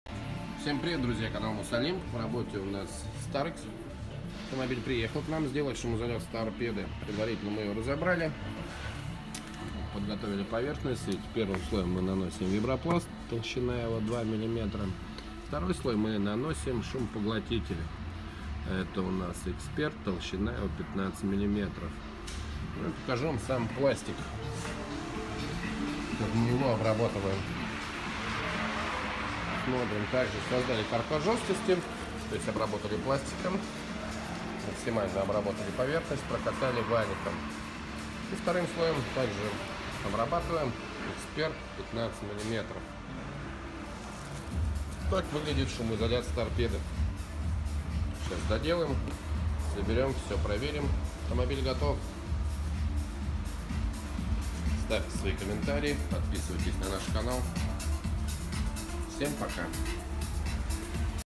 Hyundai Starex анти-скрип торпедо и вибро шумоизоляция пластика и подторпедного пространства
О полной шумоизоляции салона в следующем обзоре. А в этом видео - Hyundai Starex анти-скрип торпедо и вибро шумоизоляция пластика и подторпедного пространства.